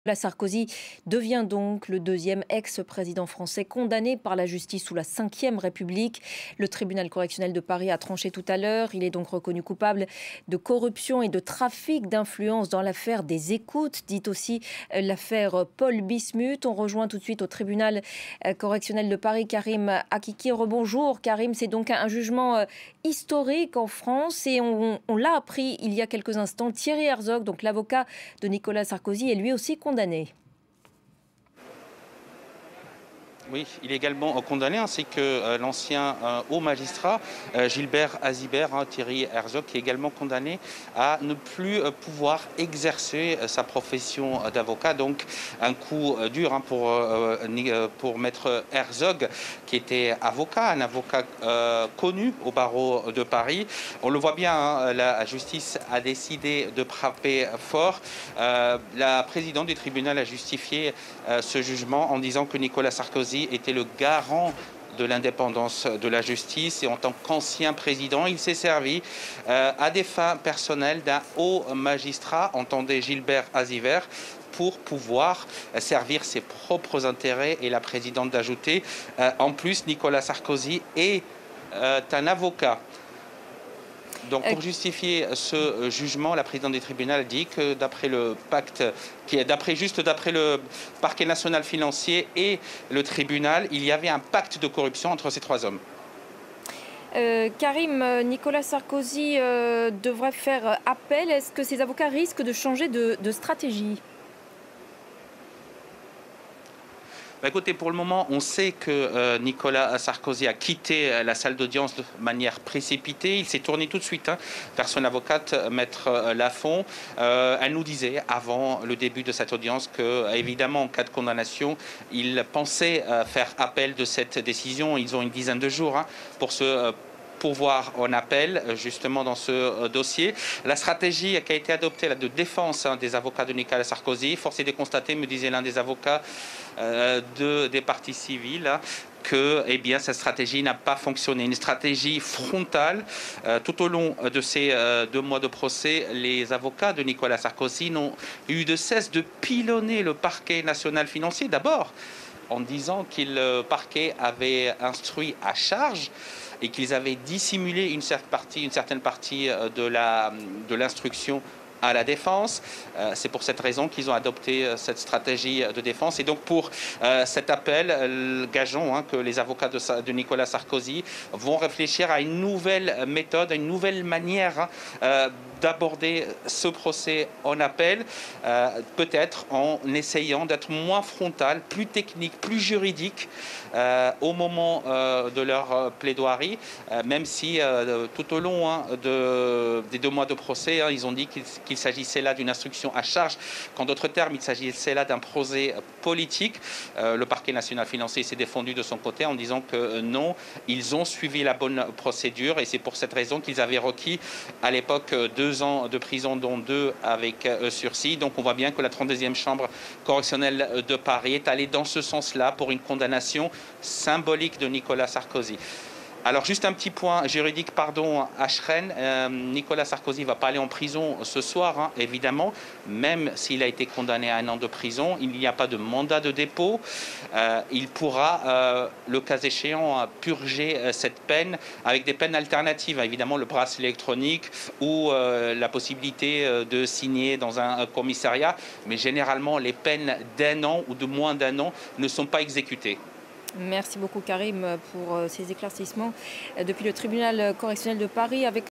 Affaire des "écoutes" : l'avocat Thierry Herzog, condamné avec Nicolas Sarkozy pour corruption
ThierryHerzog, avocat historique de NicolasSarkozy et condamné lundi comme l'ex-président à trois ans d'emprisonnement dont un ferme pour corruption et trafic d'influence dans l'affaire dite des "écoutes", a fait appel, ont annoncé ses avocats à l'AFP.